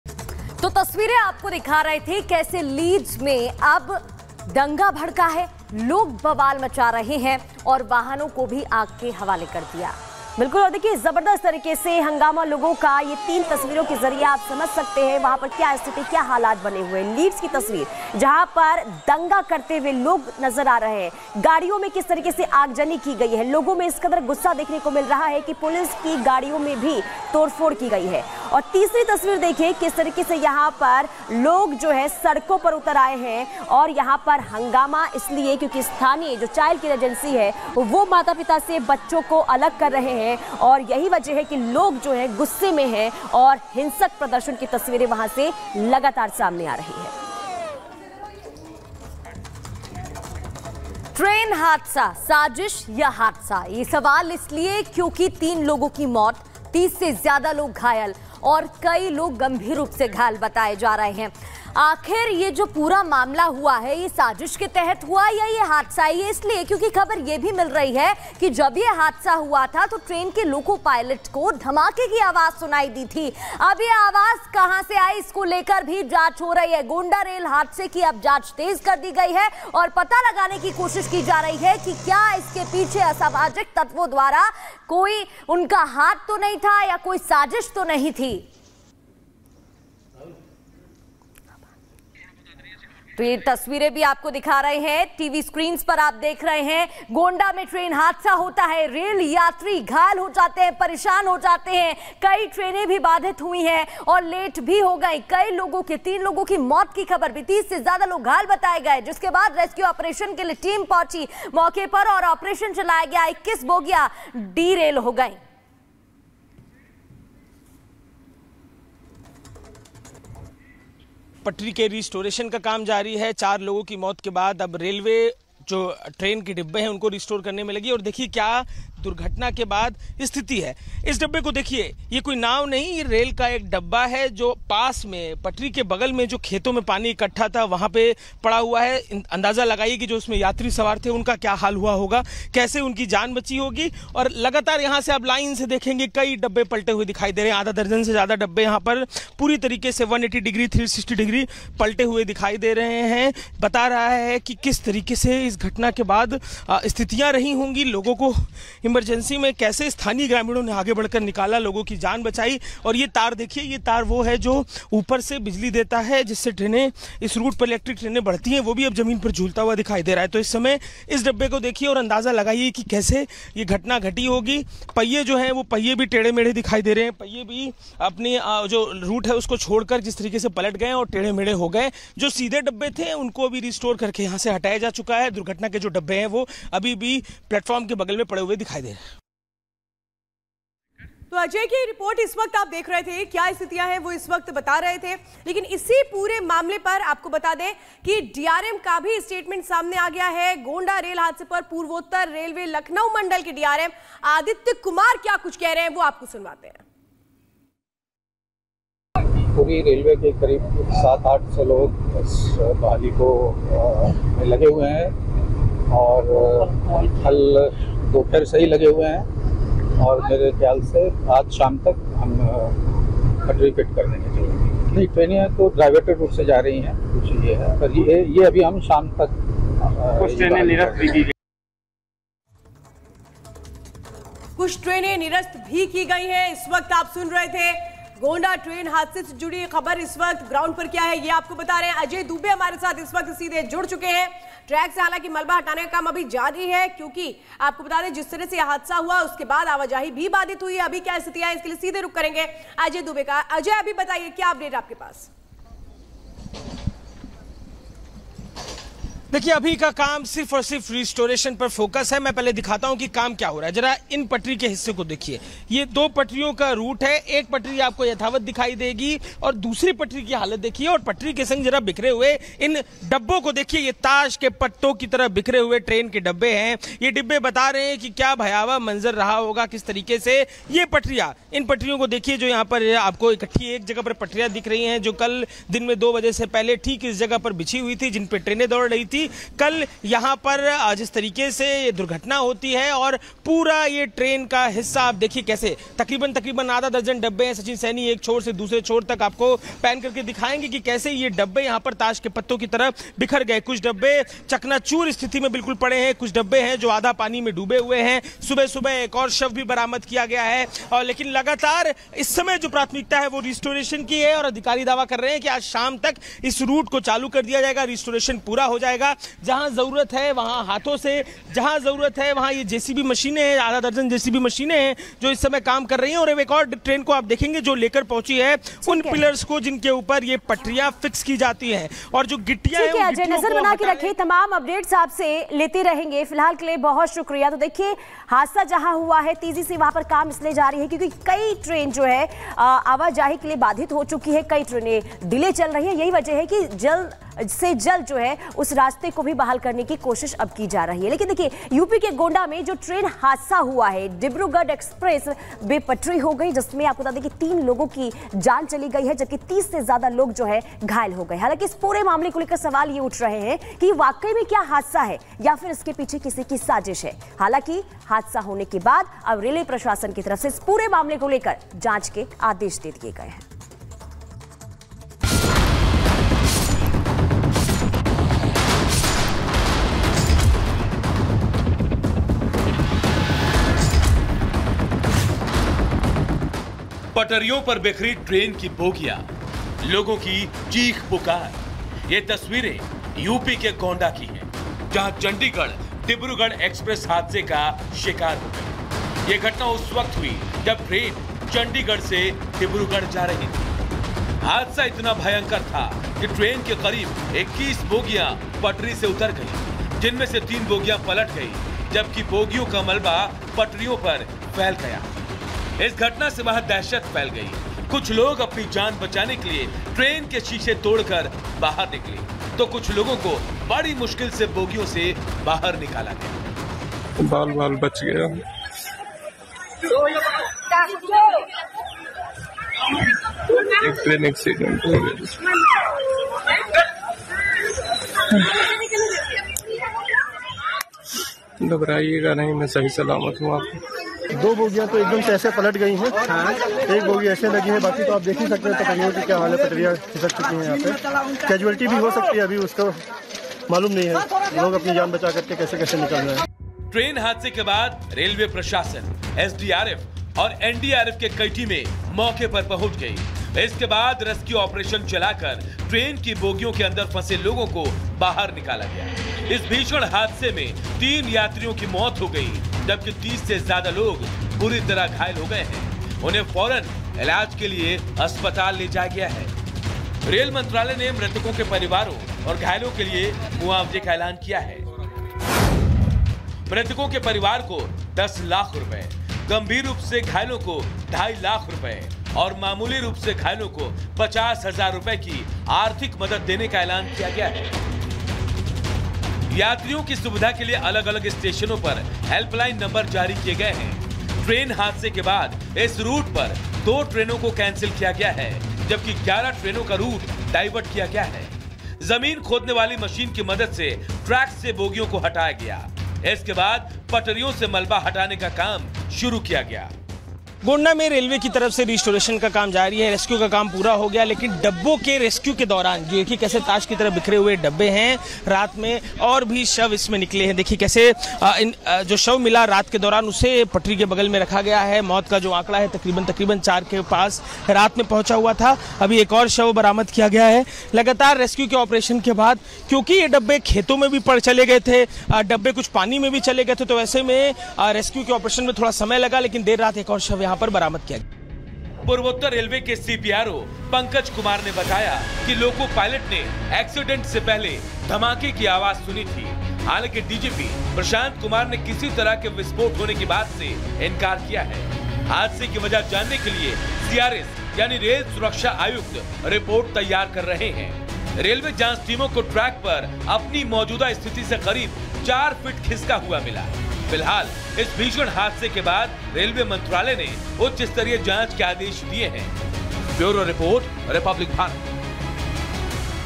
Britain Riots: Britain के लीड्स में भड़के दंगे, हिंसा से दहला देश! | R Bharat
ब्रिटेन के लीड्स में दंगा भड़क उठा है. सड़कों पर जमकर उत्पात मचाया जा रहा है. पुलिस की गाड़ियों को तोड़ा जा रहा है, जगह-जगह आगजनी की जा रही है. हिंसा की डराने वाली तस्वीरें सामने आ रही हैं.